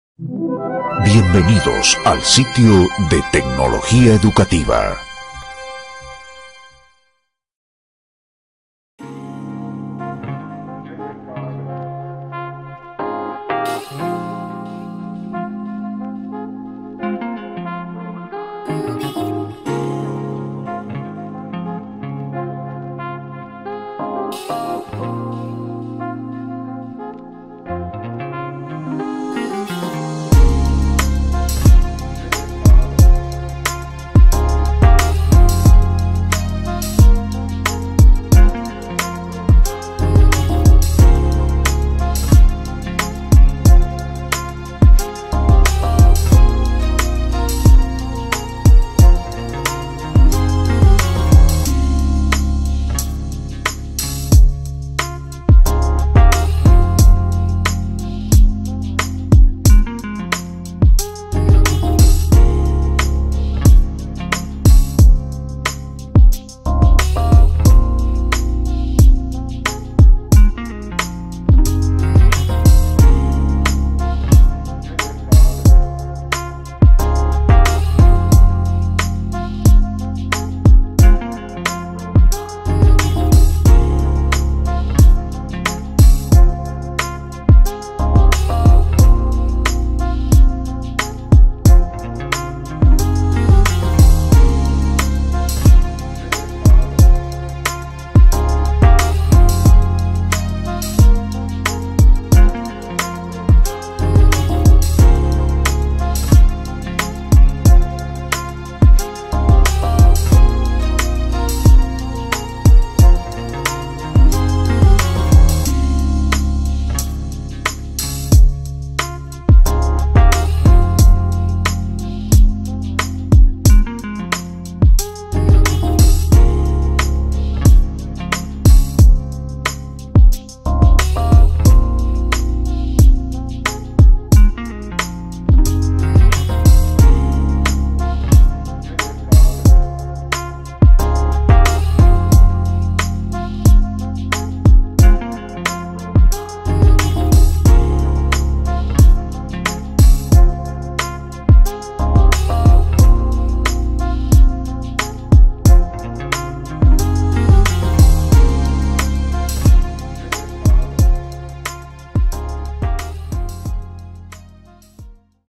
Maqueta de Torso Humano de tamaño natural con 40 piezas XC-208
Vista detallada de conjunto y despiece completo del Torso para Educación XC-208. Tamaño natural, 40 piezas, bisexuado, con útero y feto.